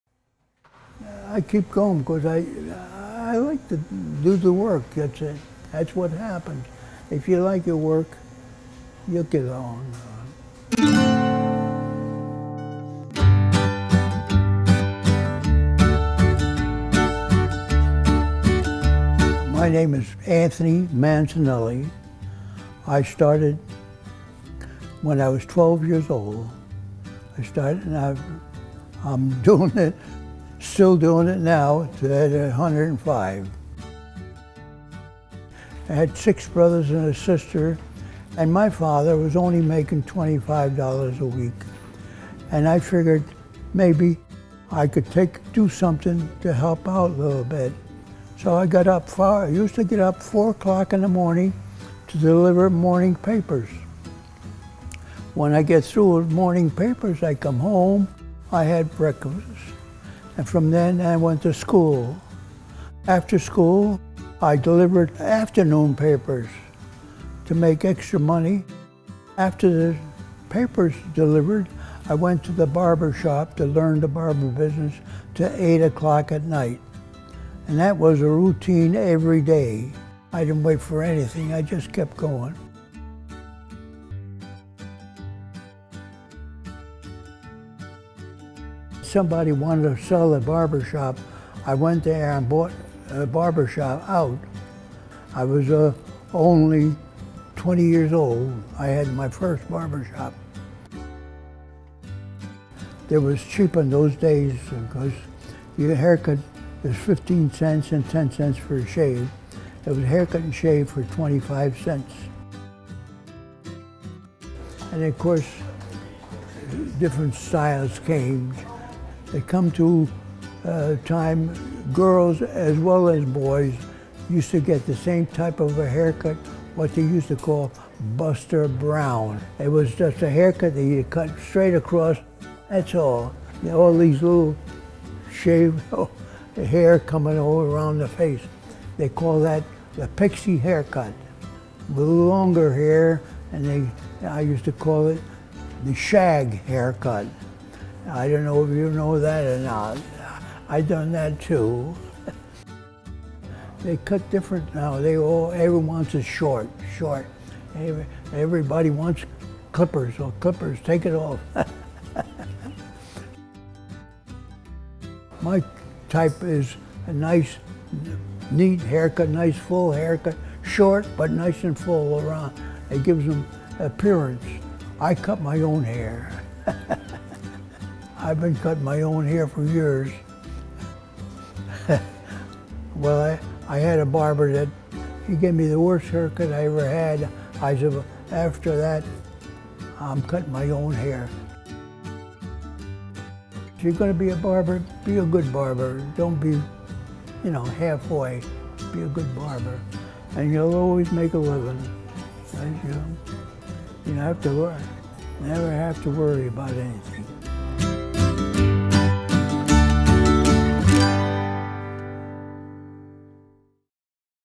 INFRINGE meets the world's oldest barber—105-year old Anthony Mancinelli
INFRINGE was honoured to meet Anthony Mancinelli, who has been barbering for 90 years… with no plans to retire